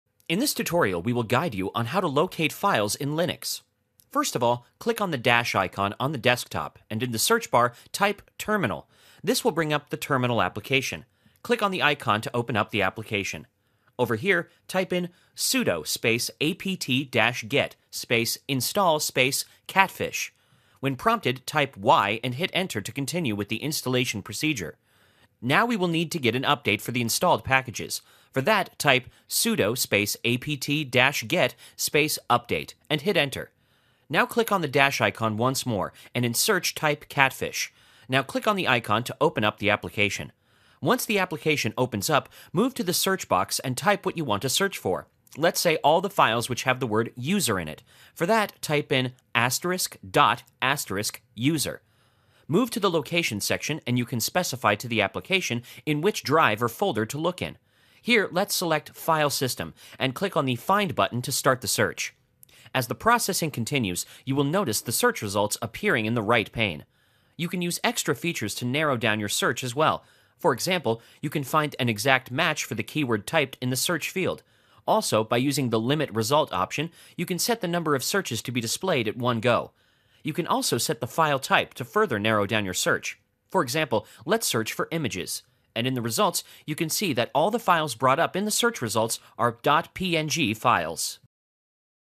How to Locate Files in Linux
In this tutorial, we will teach you how to search for and locate files in linux. Also, how you can refine your search results and apply filters

In this tutorial, we will guide you on how to locate files in Linux. We will teach you how to search for files in Linux, set search criteria. Furthermore, we will guide you on how to refine your search results and search for a specific file type.

Step 1 -- Open Terminal
First of all click on the Dash icon on the desktop and in the search bar, type "terminal". This will bring up the terminal application. Click on the icon to open up the application.

Over there, type in the following:
"sudo apt-get install catfish"

Step 2 -- Install Catfish
When prompted, type "y" and hit enter to continue with the installation procedure.

Step 3 -- Get updates
Now we will need to get an update for the installed packages. For that, type in the following: "sudo apt-get update"

Once done, hit the enter key to get the updates.

Step 4 -- Open application
Now click on the dash icon once more and in the search bar, type catfish. Now click on the icon to open up the application.

Step 5 -- Search for files
Once the application opens up, move to the search box and type what you want to search for. Let's say all the files which have the word user in it. For that, type in:
*.*user

Move to the location section and you can specify to the application in which drive or folder you want to look in. In the tutorial, we will select the File System and click on the "Find" button to start the search to locate files in linux.
As the processing continues, you will notice the search results will start appearing in the right pane. In this manner, you can locate a file in linux.

Step 6 -- Refine search
You can use extra features to narrow down your search as well. For example, you can find an exact match for the keyword typed in the search field. Also, by using the Limit Result option, you can set the number of searches to be displayed at one go.

Step 7 -- Set file type
You can also set the file type to further narrow down your search. For example, let's search for images. And in the results, you can see that all the files brought up in the search results are .png files. In this way, you can locate a file in linux.